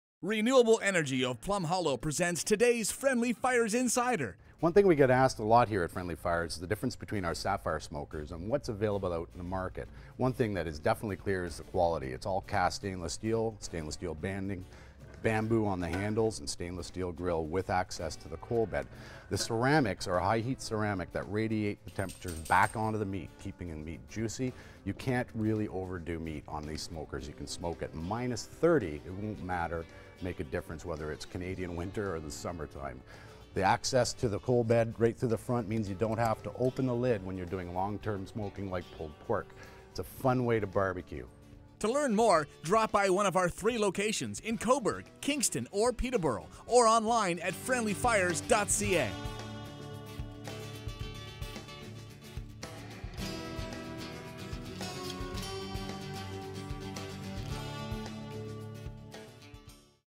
Saffire BBQ
This video shows some features of the Saffire BBQ (a competitor to the Big Green Egg / Kamado Joe / Primo / Vision Grill...). For more tips and techniques, please